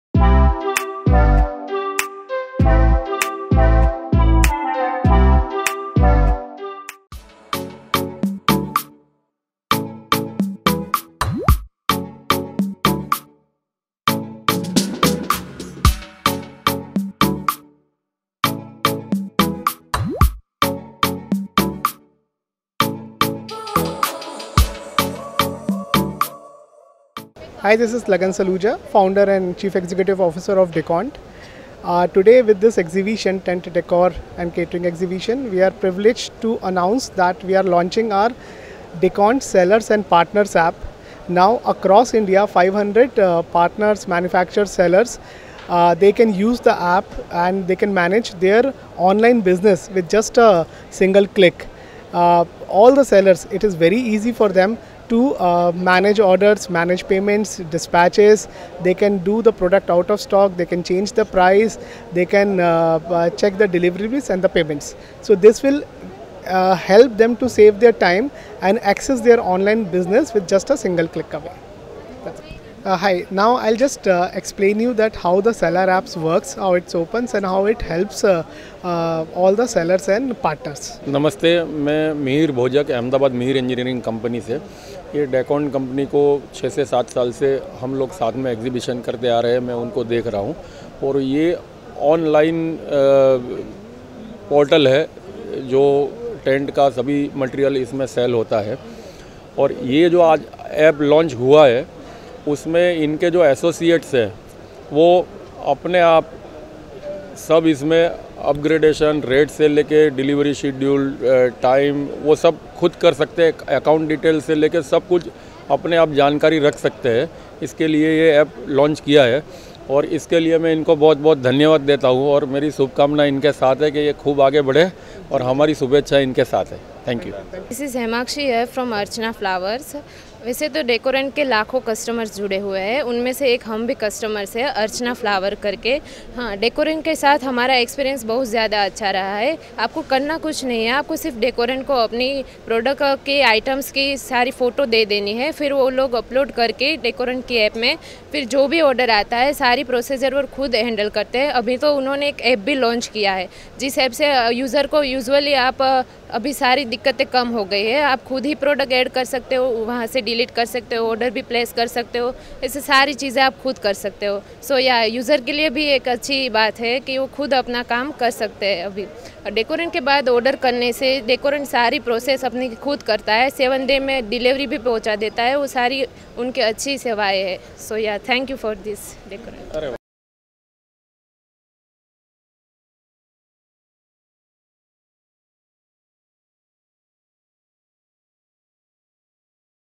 India's Largest Online Store of Decoration & Catering Material || Decornt App || Hybiz tv
TENT DECOR & CATERING-INDIA 2021 has been south India’s biggest exhibition on Tent Industry, Event Industry & Catering Industry. It has been hosted by Eagle Trade Fairs in Athidhi Conventions, Shamshabad, Hyderabad. This exhibition has showcased the latest developments & modern equipment in Tent, Decorative, Catering, Audio, Video, Lighting Event Infrastructure & Event Industry.

TENT DECOR & CATERING-INDIA 2021 is perfectly placed as the south Indian market having a lot of potential in the field, this exhibition has given a great impetus to its further diversification and growth, as well as highlighting the opportunities this region might offer. The event has attracted all the industry players, dealers, and decision-makers from the event management industry, corporate and public sectors in the region providing a strategic setting to forge business tie-ups and contacts.